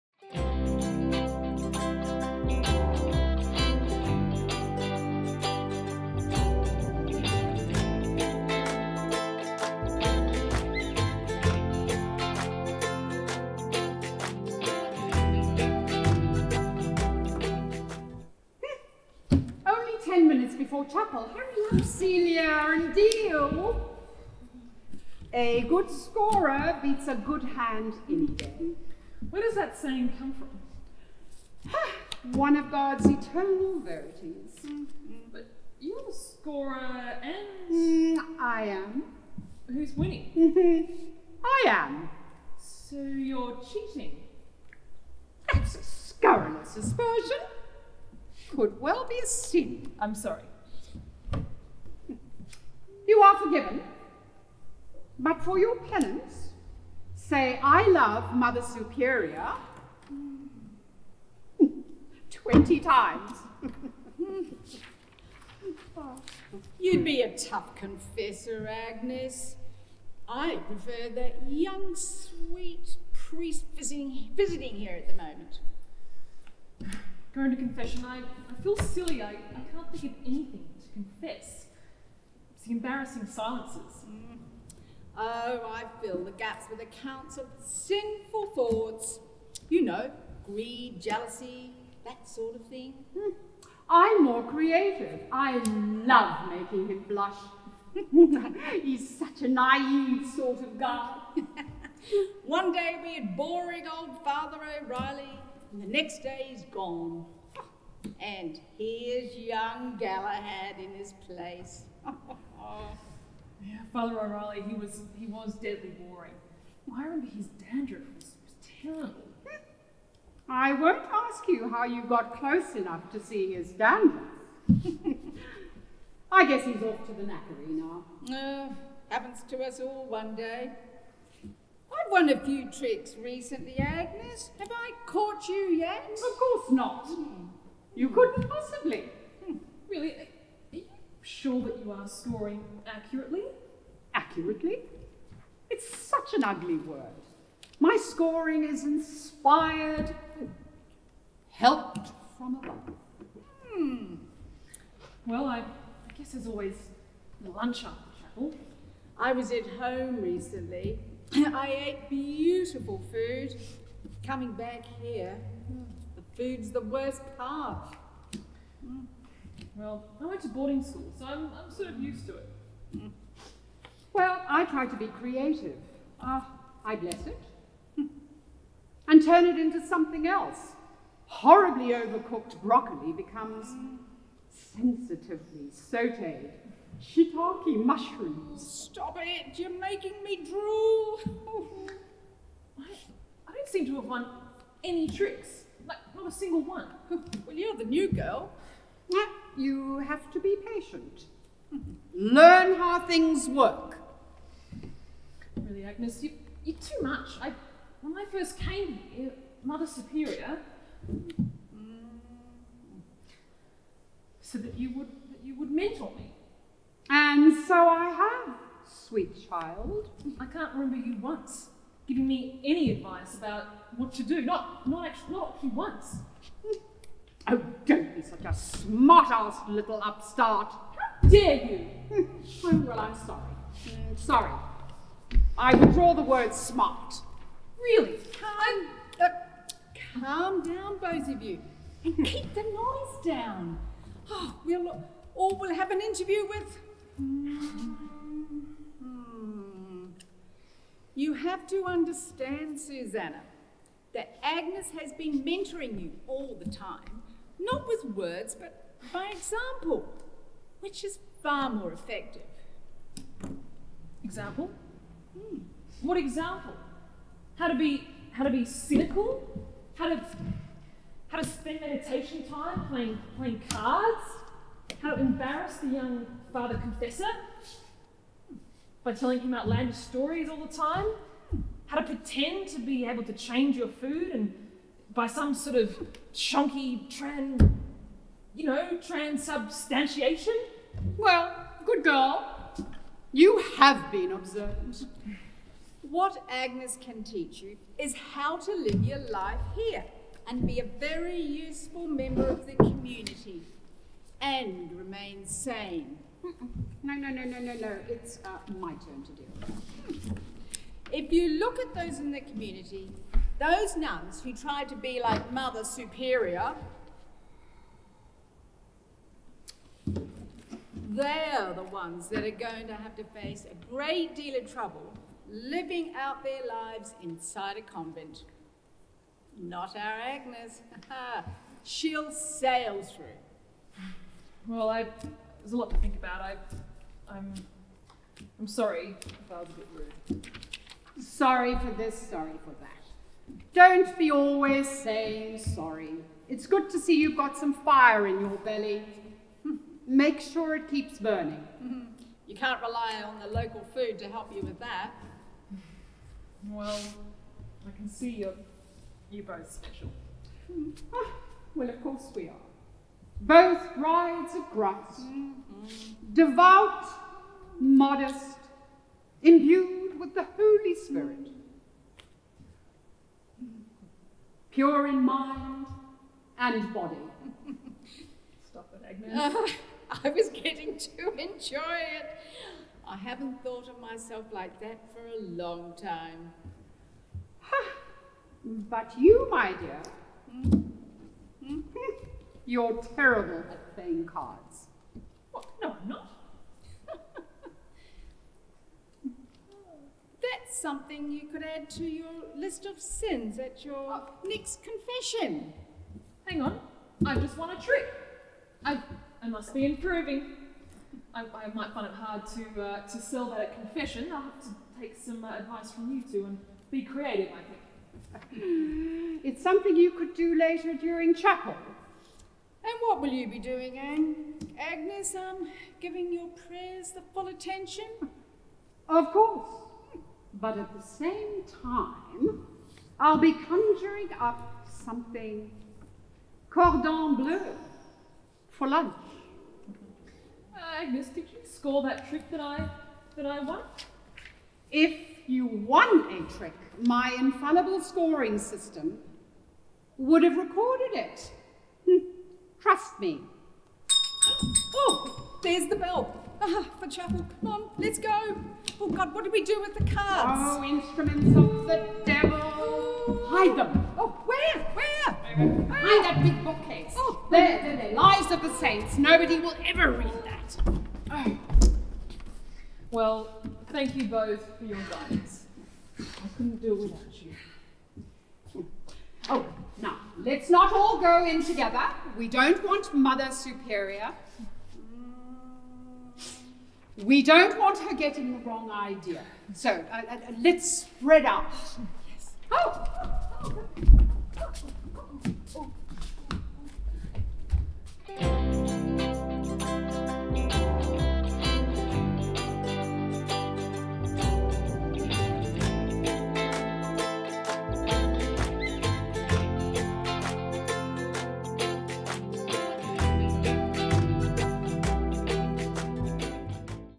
The Three Sisters
Melting Pot Theatre presents Crash Test Drama Round 3 July 2020. Crash Test is a short play competition that is performed script in hand. Normally this would be done in front of a live audience but due to COVID-19 we have moved this round of CTD online.
This is play number 2 - The Three Sisters by Greg Tome, Directed by Sarah Hawthorn. Starring Ann Poppelwell, Hollie Baillieu and Sue Ruffle. Camera and editing by Peter Gray.

MPT is a sub-committee of the Bundanoon Community Association.